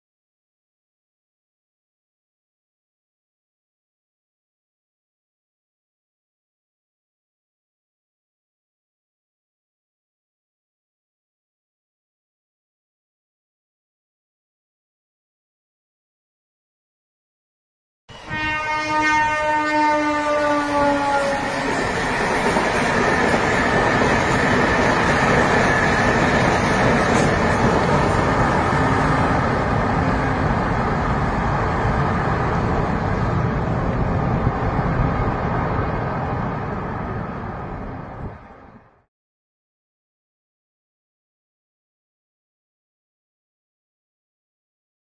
🔥🔥🔥 USBRL 100 km/h speed High Speed CRS Trials Between Reasi and Sangaldan via Chenab Bridge
high speed crs trial
chenab bridge trial run
reasi high speed trial